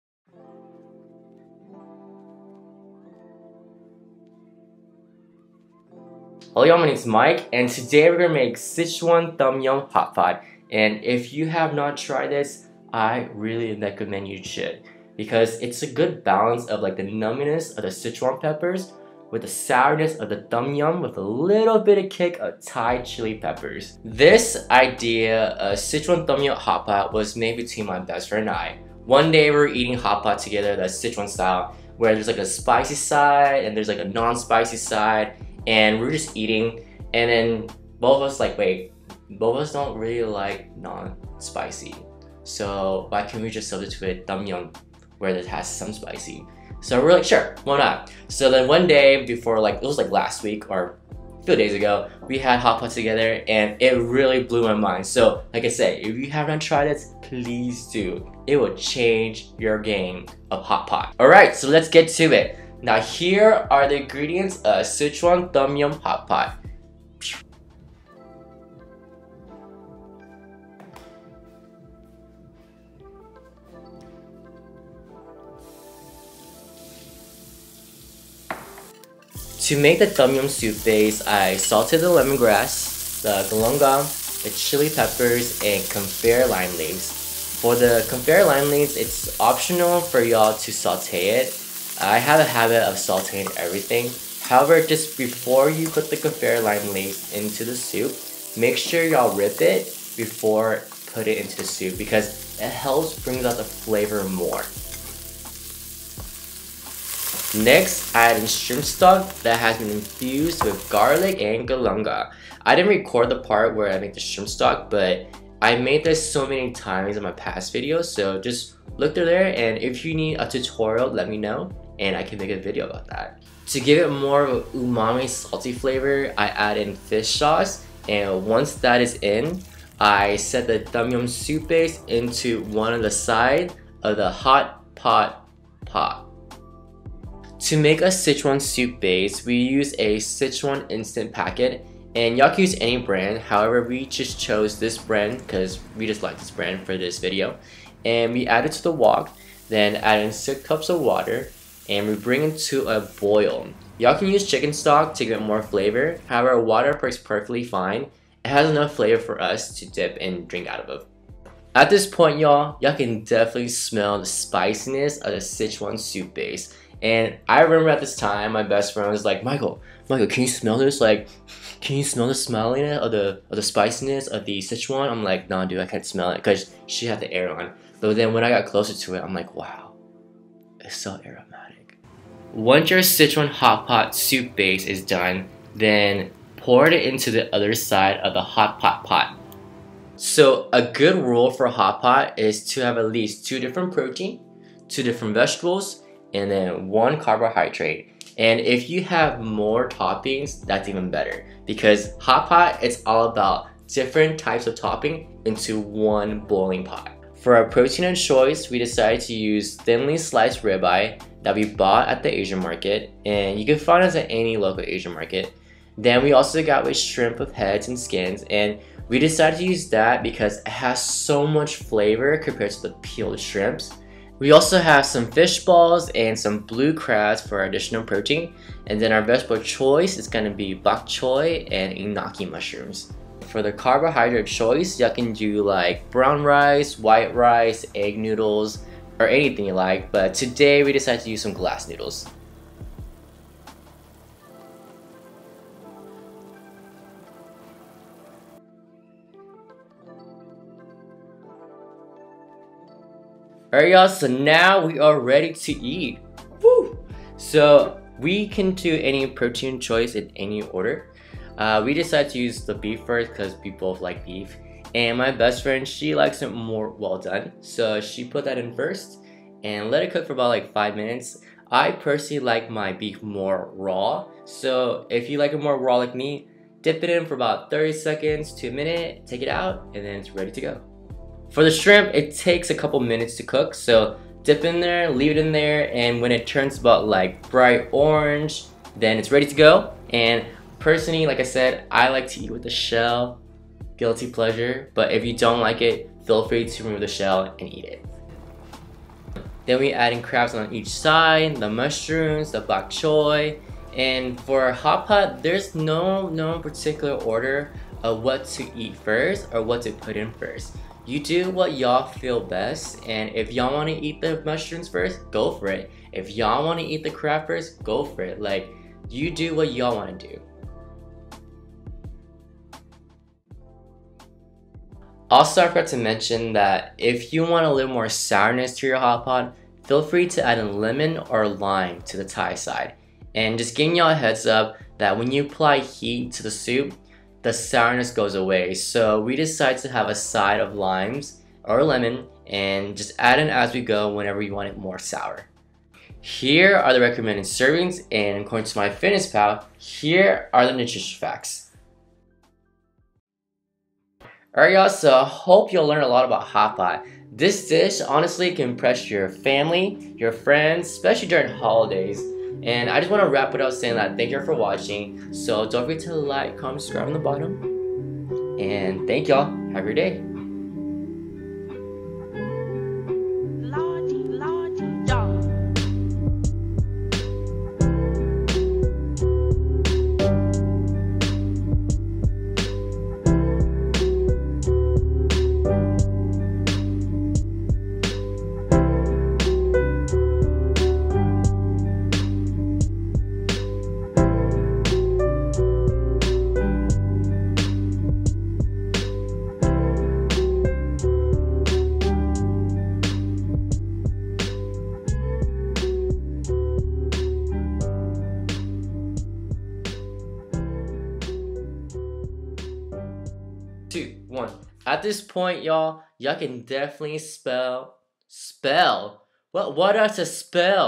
Sichuan Tom Yum (Asian Fusion) Hot Pot Recipe
Recipe
Tom Yum
6 Cups of Shrimp Stock
2 Stalks of Lemon Grass
10 Kaffir Lime Leaves
1 Inch Galangal
3 Tbsp Fish Sauce

Sichuan Hot Pot
6 Cups of Water or Chicken Stock
1 Sichuan Seasoning Packet